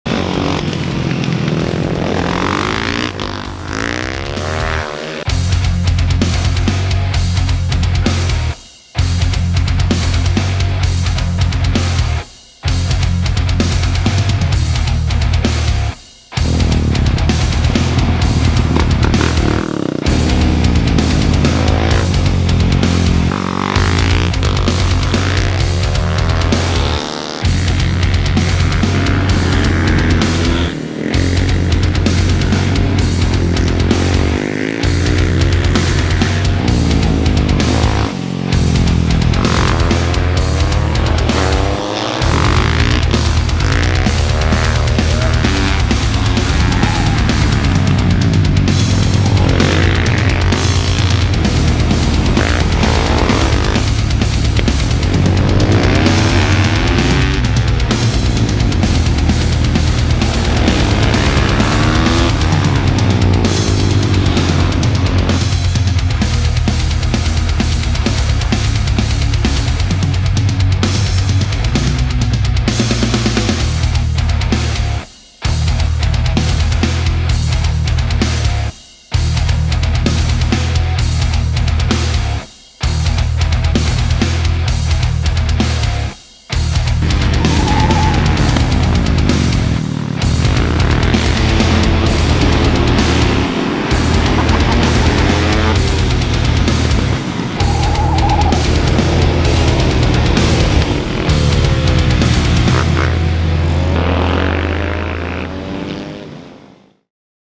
Jacks Fast CTECH Manufacturing Motorplex Supermoto Race
Ct Manufacturing Motorplex Supermoto Races At Road America Elkhart Lake Wisconsin
Jack Jepson Highlights